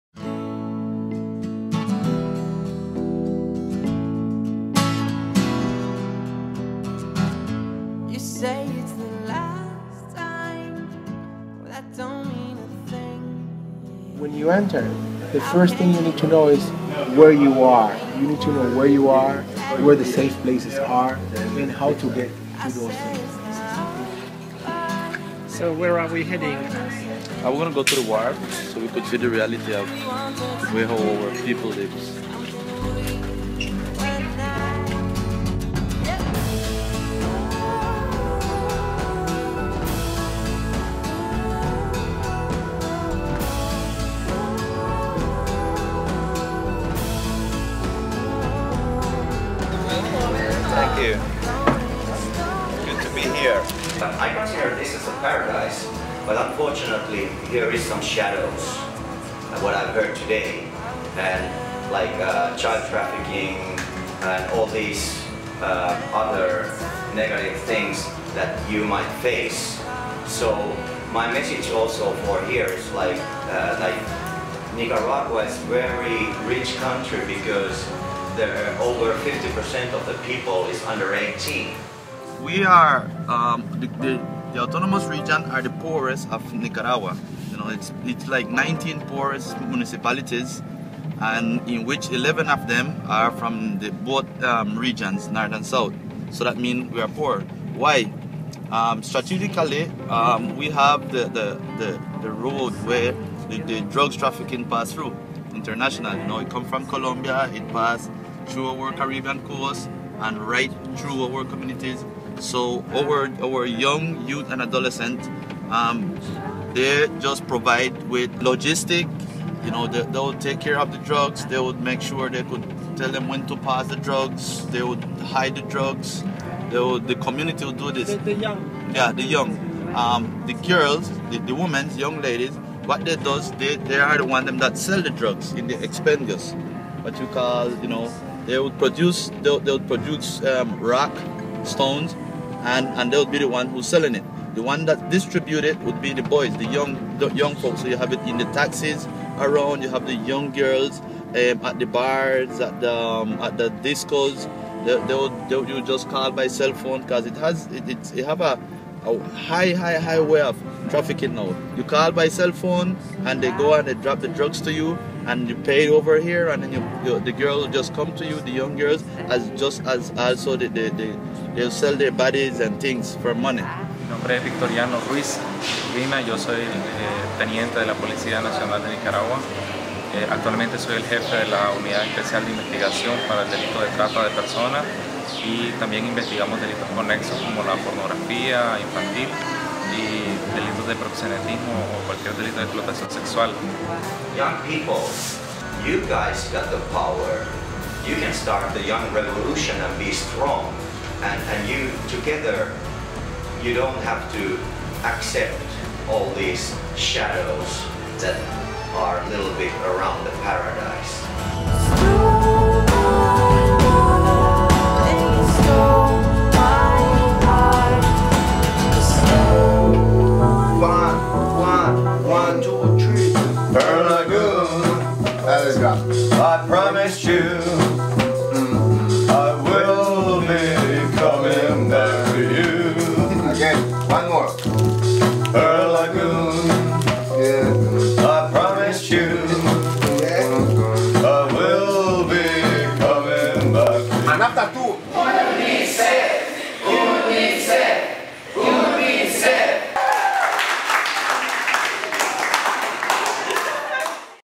Jyrki69 in Nicaragua
The Good Will Ambassador of Finnish UNICEF, Jyrki69 from the band The 69 Eyes visits Nicaragua to fight child abuse. Video by Estevan Oriol.